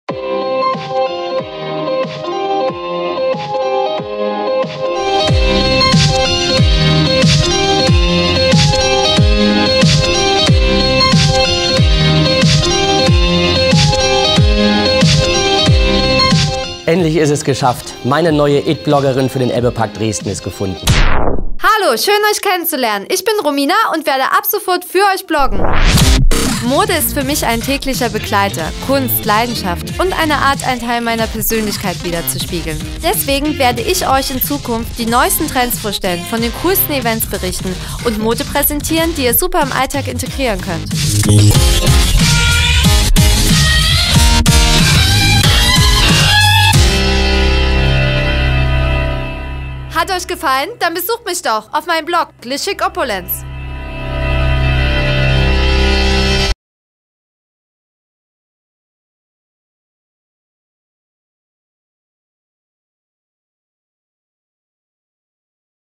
Romina Bloggerin für den ELBEPARK Dresden
Hallo liebe Freunde, wir möchten euch Romina vorstellen, die wir als Bloggerin unterstützen und die ihr ab sofort auch regelmäßig im ELBEPARK Dresden treffen werdet. Also schaut mal rein, folgt ihr und freut euch auf ihre Fashion- und Event-Empfehlungen – in ihrem Blog und in unserer Online-Welt „Trends for Friends“.